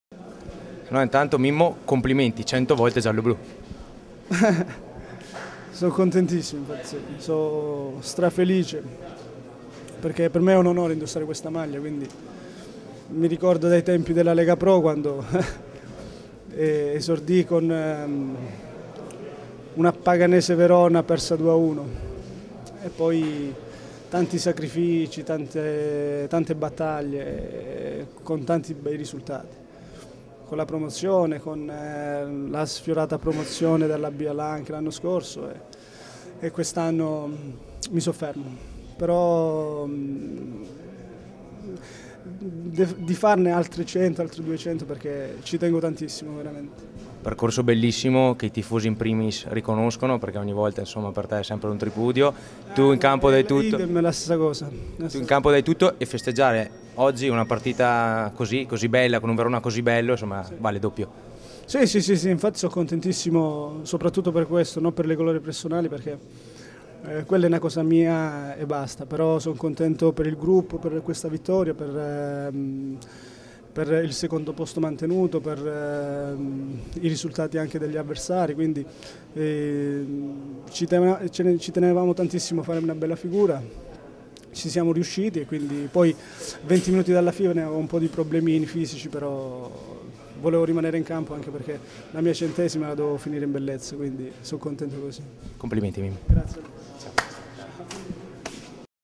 6 aprile 2013 - Domenico Maietta
Il difensore gialloblù al termine della sfida con la Ternana: "Sono onorato di aver raggiunto le 100 presenze con questa maglia, spero di farne almeno altrettante. Quando guardo indietro vivo un sacco di emozioni diverse".